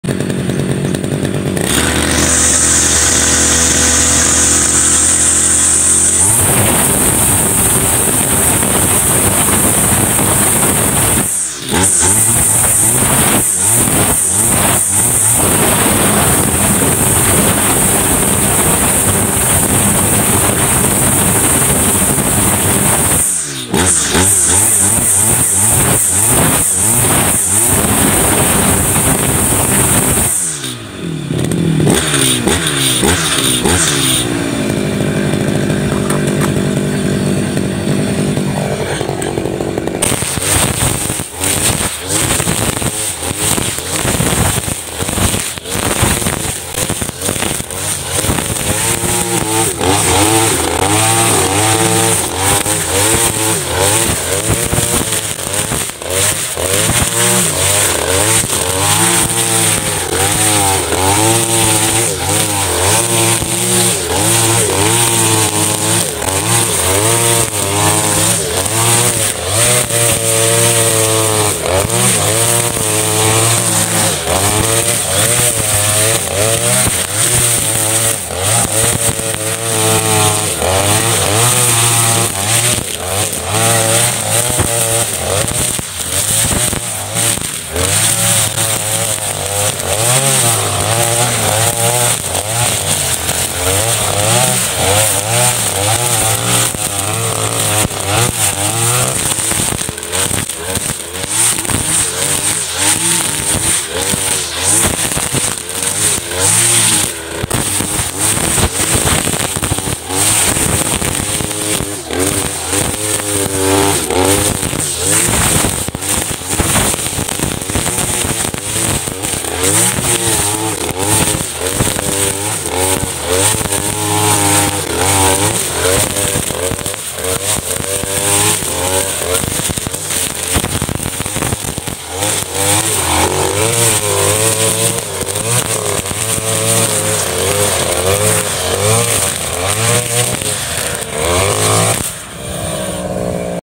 Orang ini Terlalu Ogros Dalam Mengendalikan mesin Potong rumput
sangat mengerikan orang ini terlalu Ogros dalam mengendalikan mesin potong rumput